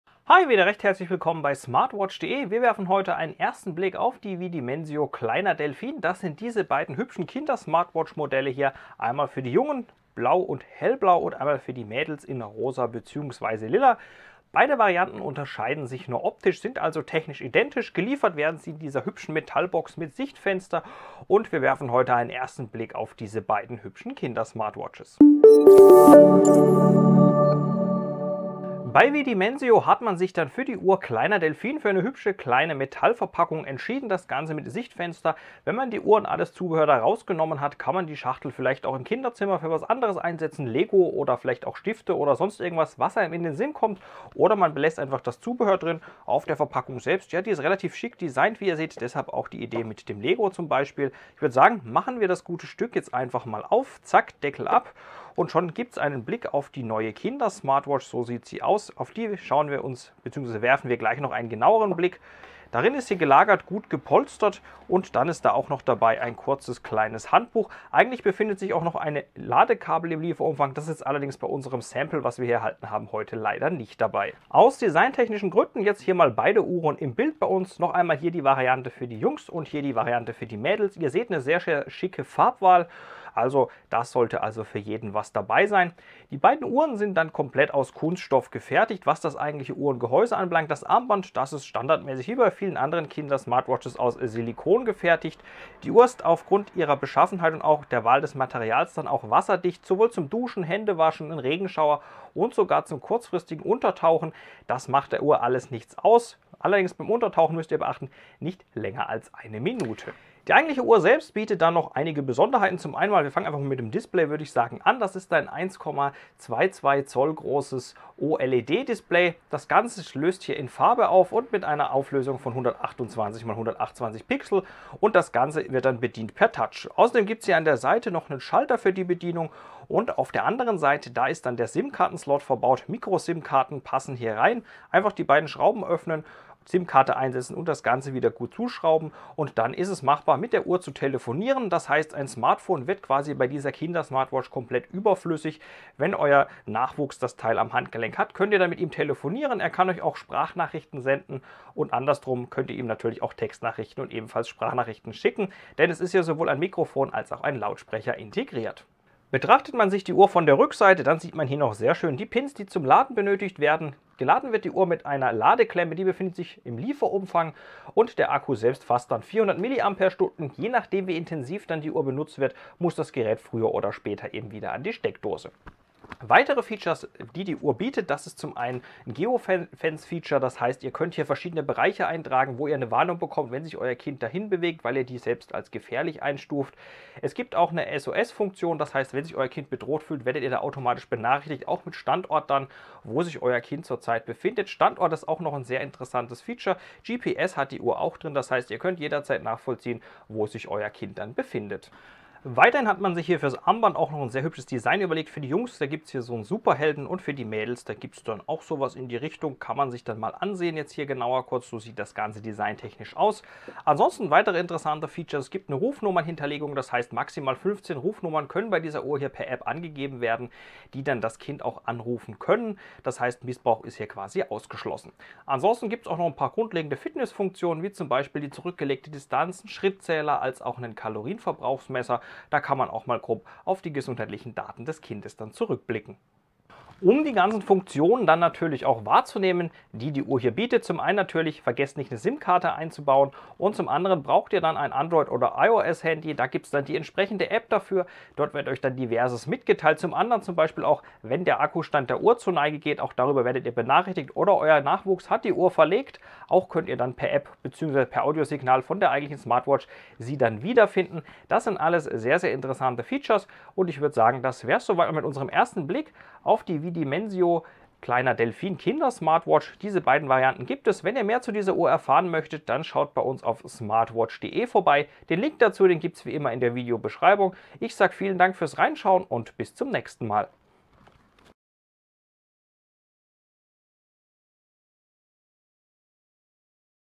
Vidimensio Kleiner Delfin - Smartwatch.de Unboxing [DEUTSCH]
Heute beim uns im Unboxing - Die Vidimensio Kleiner Delfin.

Der kleine Delfin ist eine in zwei Farbausführungen erhältliche Kindersmartwatch aus dem Hause Vidimensio, welche nicht nur ein unpraktisches Handy erübrigt, sondern auch zahlreiche Features bietet. Eine SOS Funktion, Sprachnachrichten, Schrittzähler und Wecker machen nur eine Auswahl der zusätzlichen Funktionen des kleinen Delfins aus. Hinzu kommt eine altersgerechte Bedienung der Smartwatch, deren kompakte Maße und angenehmes Gewicht, sowie eine benutzerfreundliche App für iOS und Android Smartphones der Eltern.